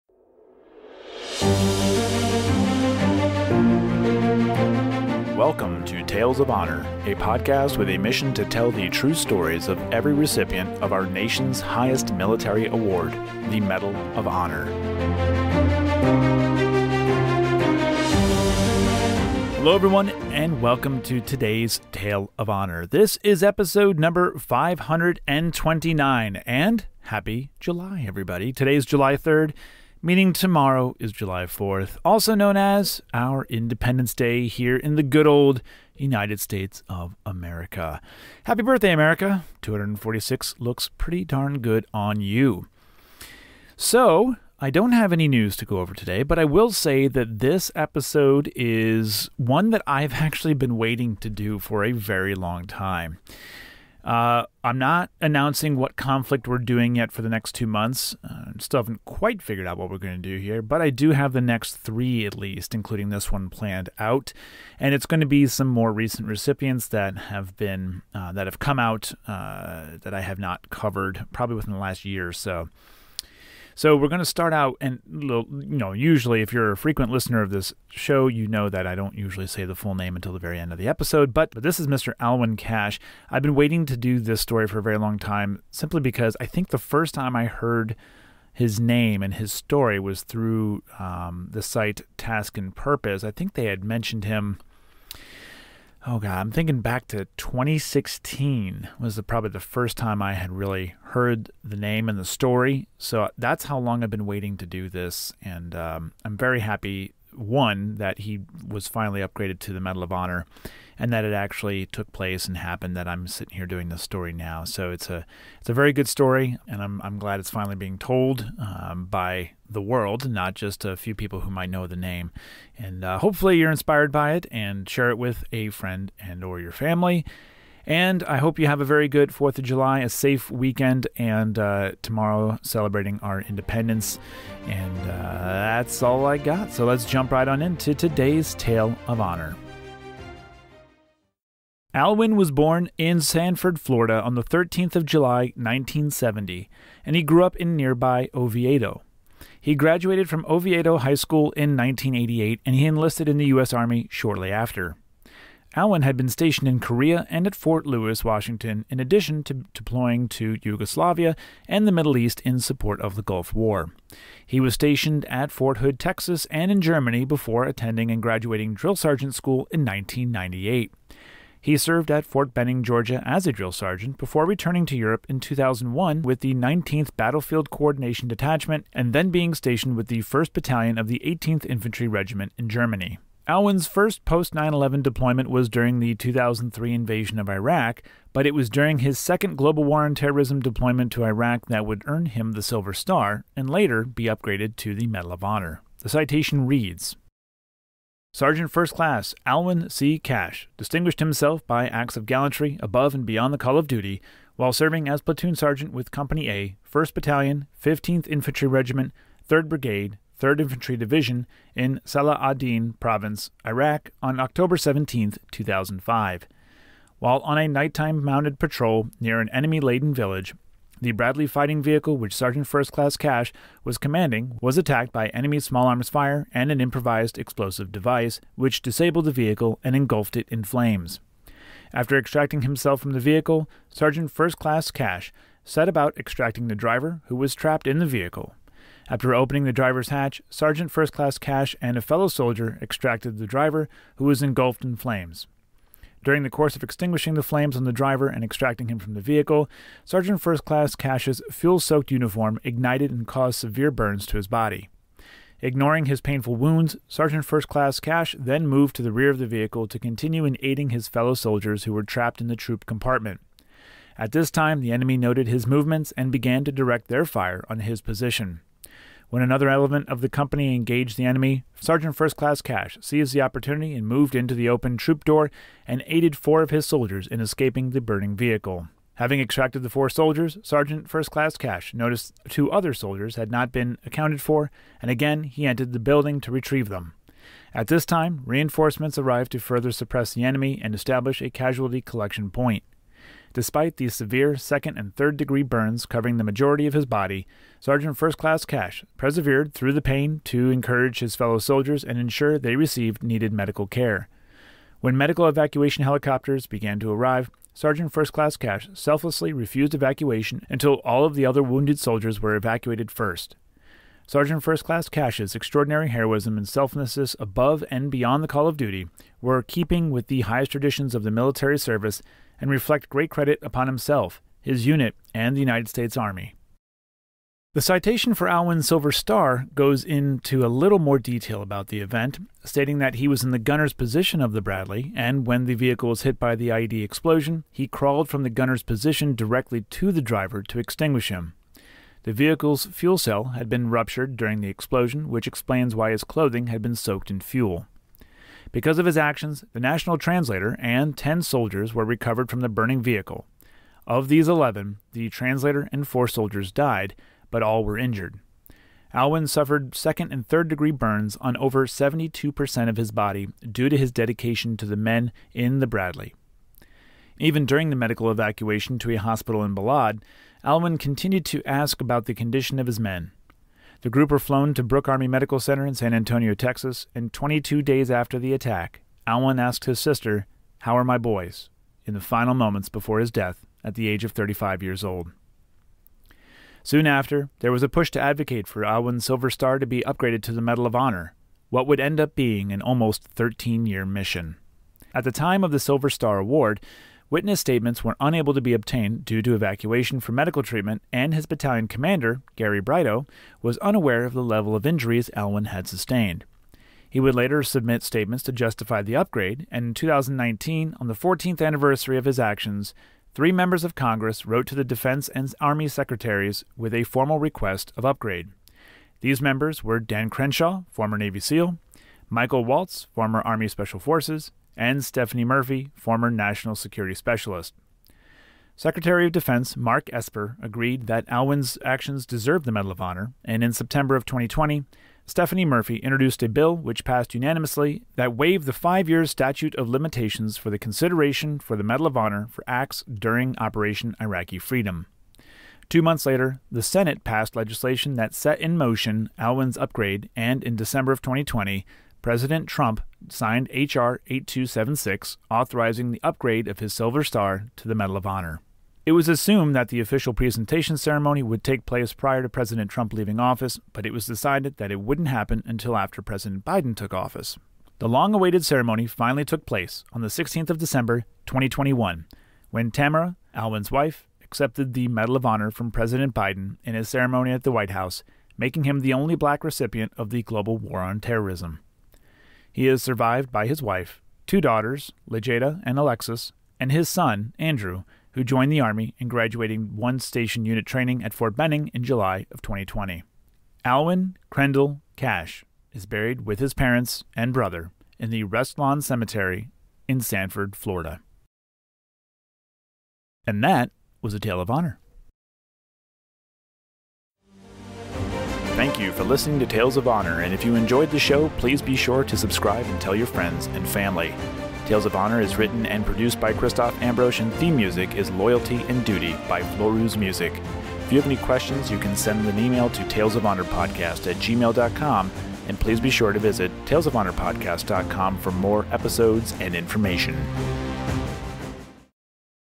529. Alwyn C Cashe - Medal of Honor Recipient
On episode five hundred and twenty-nine, the story of Alwyn C Cashe is told.

A big thank you goes out to this month's show sponsor, Motor City Munitions. All your firearm related needs with service so fast it can only come from the Motor City